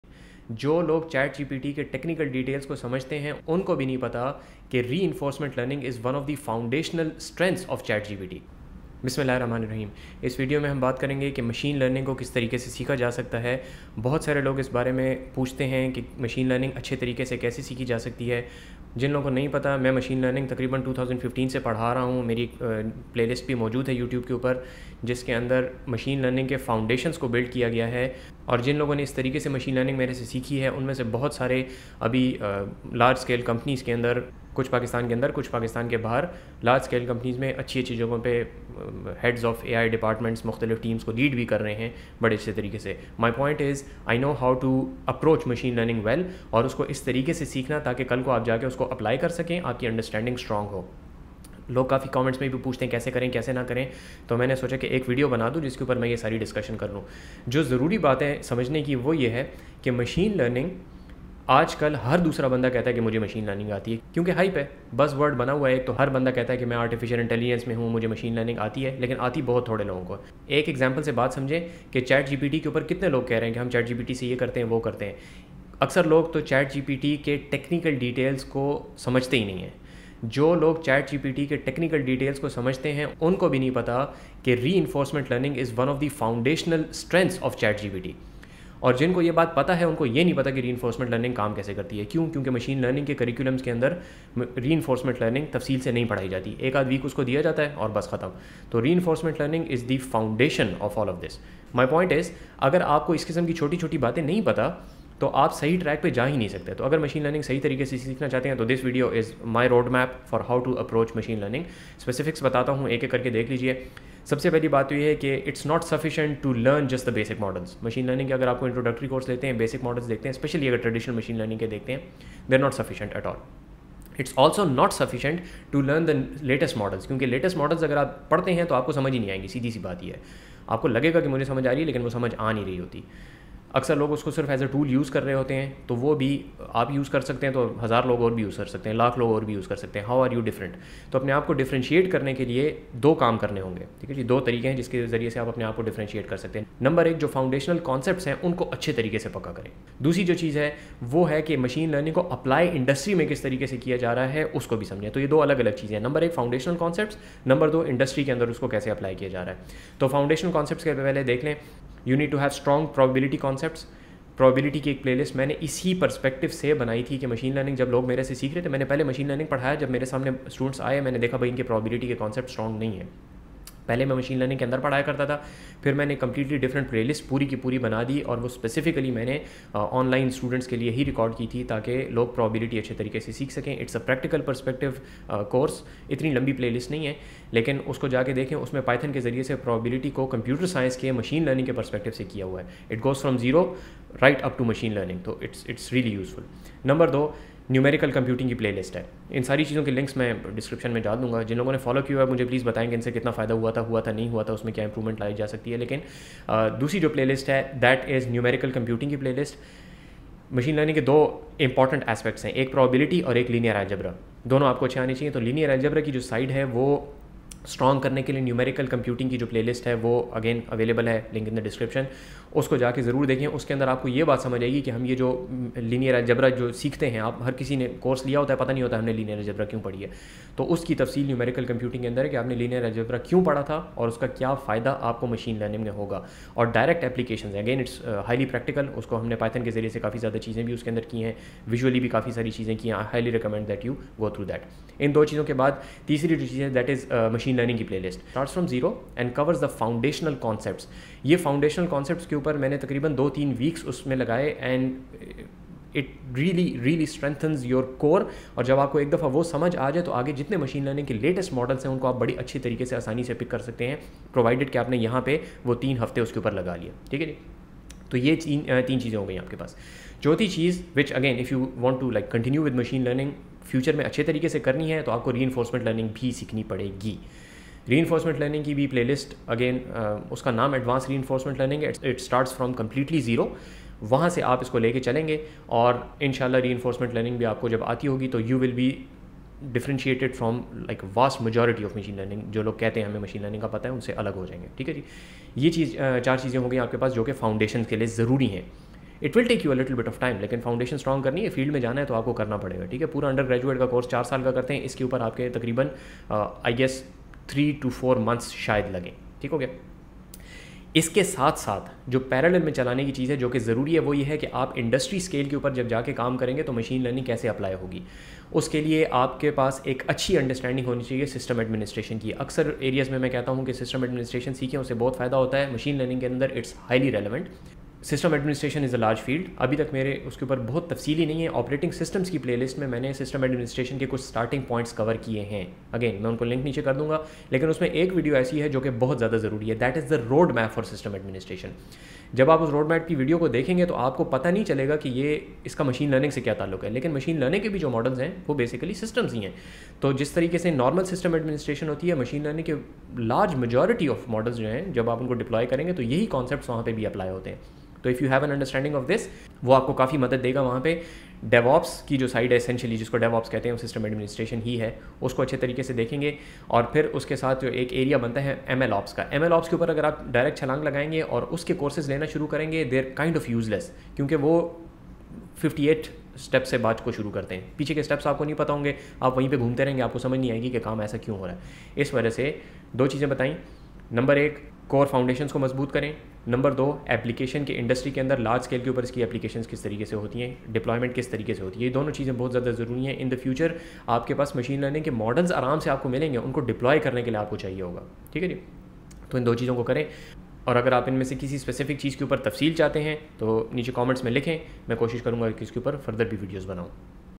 Machine Learning Full Roadmap | Learn ML the Right Way!
In this video, I discuss the roadmap for learning "Machine Learning" effectively by referring to my playlists.

0:00 Why would you trust my roadmap?
1:11 People don't really know machine learning
1:54 Two important rules for learning AI/ML
2:39 Foundational concepts
4:29 Reinforcement learning
5:10 System administration / ML in the industry

I cover the following topics in detail:

- Probability playlist: This playlist covers the basics of probability theory practically using Python.

- Numerical methods for linear algebra playlist: This playlist covers the numerical methods used in linear algebra and their use in ML models.

- Machine learning playlist: covering the fundamentals of machine learning core concepts.

- Reinforcement learning playlist: This playlist covers the basics of reinforcement learning, which is essential to set you apart from everyone else claiming to be a machine learning expert.

- A roadmap for learning system administration and getting you started with MLOps.

By the end of this video, you will have a clear understanding of the roadmap for learning machine learning in an effective way by referring to my playlists. I hope you find this video helpful and informative. Good luck with your learning journey!